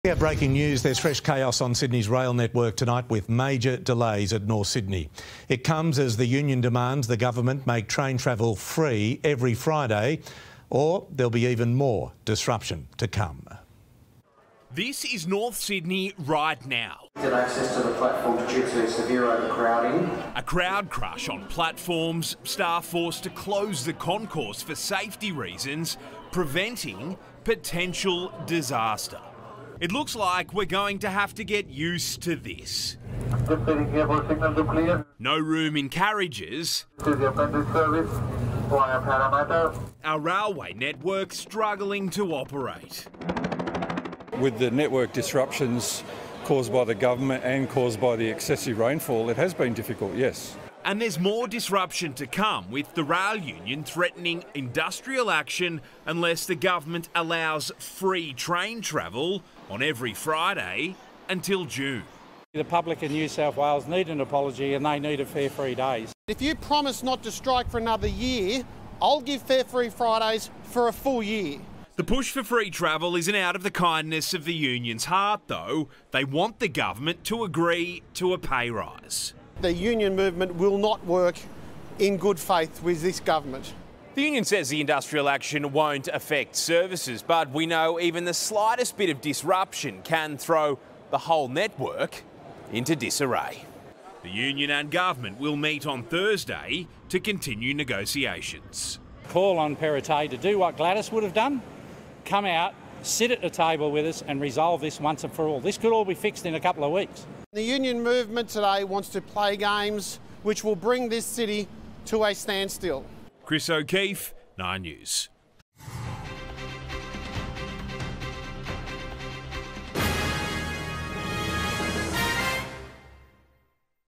Calls for 'fare-free Friday' as union threatens to 'force' industrial action | 9 News Australia
Commuters could receive free fares on Sydney trains every Friday until June as the NSW Rail, Tram and Bus Union threatens to ‘force’ industrial action. It comes as a recent “communication failure” and wet weather caused major train delays during peak hours, leaving some commuters stuck on immobilised trains for over an hour.